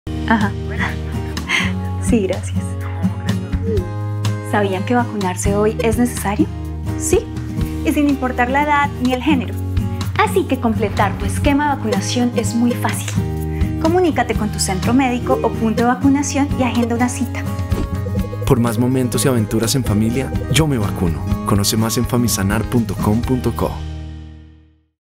¡Vacunarse hoy es necesario!
Sin importar la edad ni el género, completar tu esquema de vacunación es muy fácil.

Comunícate con tu centro médico o punto de vacunación y agenda una cita.